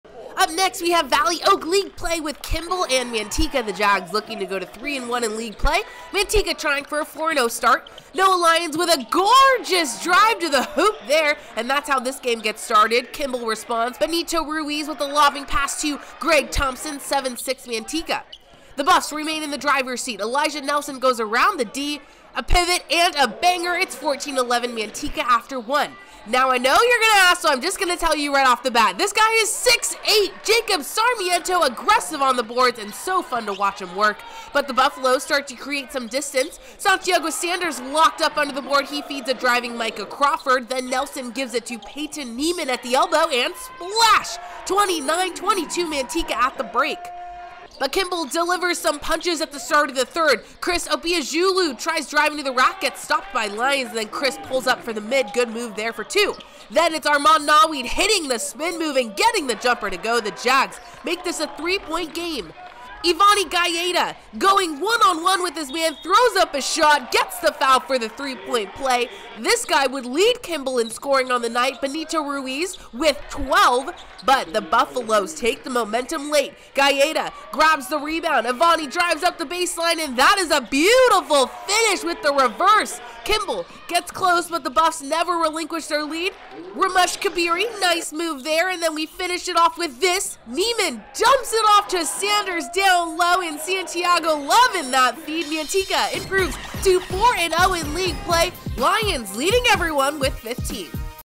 Kimball at Manteca Boys Basketball 1.18.23
UP NEXT WE HAVE VALLEY OAK LEAGUE PLAY WITH KIMBALL AND MANTECA. THE JAGS LOOKING TO GO TO 3-1 IN LEAGUE PLAY, MANTECA TRYING FOR A 4-0 START.
NOAH LYONS WITH A GORGEOUS DRIVE TO THE HOOP HERE. AND THATS HOW THIS GAME GETS STARTED.
KIMBALL RESPONDS. 14 BENITO RUIZ WITH THE LOBBING PASS TO 13 GREG THOMPSON. 7-6 MANTECA.
THE BUFF REMAIN IN THE DRIVERS SEAT. ELIJAH NELSON GOES AROUND THE DEFENSE... A PIVOT AND A BANGER. 14-11 MANTECA AFTER ONE.
NOW I KNOW YOU'RE GOING TO ASK SO I'M JUST GONNA TELL YOU RIGHT OFF THE BAT. THIS GUY IS SIX-EIGHT! JACOB SARMIENTO AGRESSIVE ON THE BOARDS. SO FUN TO WATCH HIM WORK!
BUT THE BUFFALOES START TO CREATE SOME DISTANCE. SANTIAGO SANDERS LOCKED UP0 UNDER THE BOARD.,.. HE FEEDS A DRIVING 21 MICAH CRAWFORD.
THEN 12 NELSON GIVES IT TO 3 PEYTON NIEMAN AT THE ELBOW AND SPLASH. 29-22 MANTECA AT THE BREAK.
BUT KIMBALL DELIVERS SOME PUNCHES AT THE START OF THE THIRD. CHRIS OBIAJULU TRIES DRIVING TO THE RACK.. GETS STOPPED BY LYONS AND THEN CHRIS PULLS UP FOR THE MID. GOOD MVOE FOR TWO THERE.
THEN ITS ARMON NAWEED HITTING THE SPIN MOVE AND GETTING THE JUMPER TO GO. THE JAGUARS MAKE THIS A THREE POINT GAME.
EVANI GAETA GOING ONE ON OPNE WITH HIS MAN... THROWS UP THE SHOT AND GETS THE FOUL FOR THE THREE POINT PLAY.
THIS GUY WOULD LEAD KIMBALL IN SCORING ON THE NIGHT. BENITO RUIZ... WITH 12.
BUT TGHE BUFFALOS TAKE THE MOMENTUM LATE...  GAETA GRABS THE REBOUND... EVANI DRIVES UP THE BASERLINE AND THAT IS A BEAUTIFUL FINISH THERE WITH THE REVERSE.
KIMBALL GETS CLOSE BUT THE BUFFS NEVER RELIQUISH THEIR LEAD. RAMESH KABIRI NICE MOVE THERE.
AND WE FINISH IT OFF THIS NIEMAN... DUMPS IT OFF TO SANDERS DOWN LOW... AND SANITAGO LOVING THAT FEED. MANTECA IMPROVES TO 4-0 IN LEAGUE PLAY.